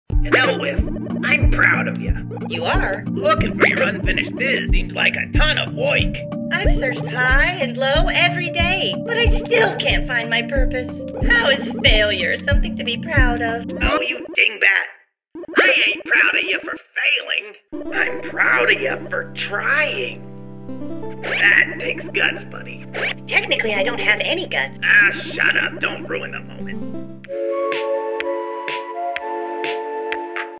Dungeon Follies 20 | “Whiff Is A Tryhard”
What have you been trying to do lately?

I really let myself enjoy the process on making this one. The lighting and color grading were especially fun to play with. This idea came from my desire to sit on my own private cloud with my friends. These animations have been so therapeutic to create and I find myself using them as a means of working through things. So, in a way, this episode is me talking to myself (especially since I do all the voices). I hope you send this one to someone who’s been trying hard. Or, better yet, tell them yourself. I’m sure you will absolutely make their day. -Erk
- Script, art, animation, & voices by Eric T. Martin.
Music, SFX, & extra images courtesy of opengameart.org & Pixabay.
Theme music by Alex McChulloch.